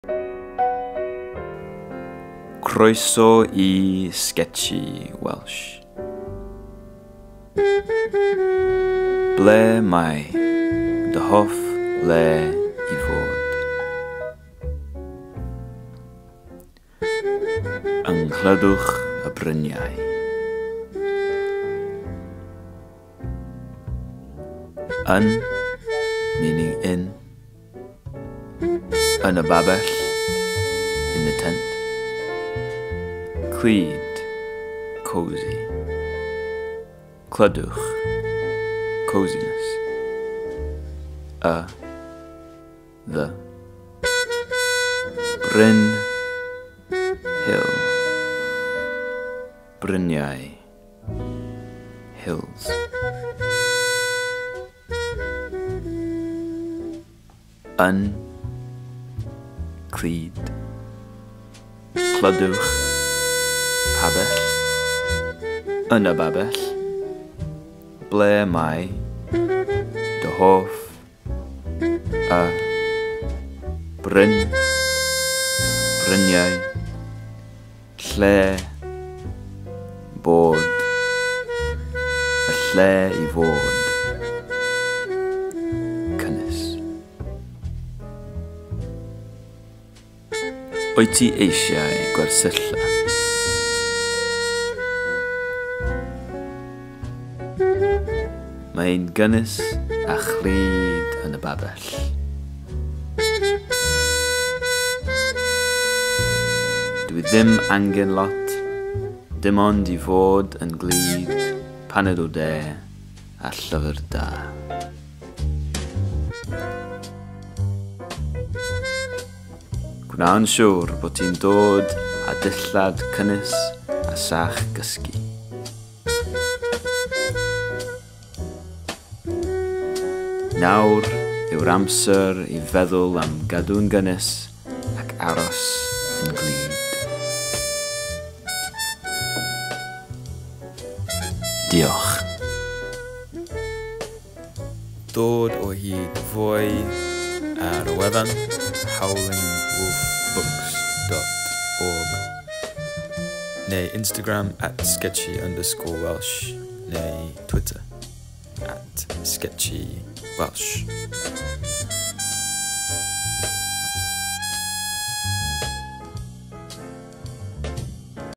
Sketchy Welsh - Your Favourite Place - a short lesson yn Gymraeg
How to ask “where is your favourite place to be” yn Gymraeg (in Welsh)

If you would like a print of this piece you can find it here!:

‘Yng nghlydwch y bryniau’: In the safety of the hills.

Clydwch: means shelter/safety/warmth/cosiness, perhaps put it in your mind next to the word ‘cwtch’.
yn: in, bryn: hill, bryniau: hills,
 Ble mae dy hoff le i fod: Where is your favourite place to be?
 ble: where
dy: your
hoff: favourite
lle: place
 lleoedd: places
bod: to be

@this.girlwalks (instagram) has written a blog about being ‘in the safety of the hills’. It’s great. Follow the links on her page!

Mae @this.girlwalks wedi ysgrifennu blog am fod ‘Yng nghlydwch y bryniau’. Mae’n wych. Dilynwch y dolenni ar ei thudalen!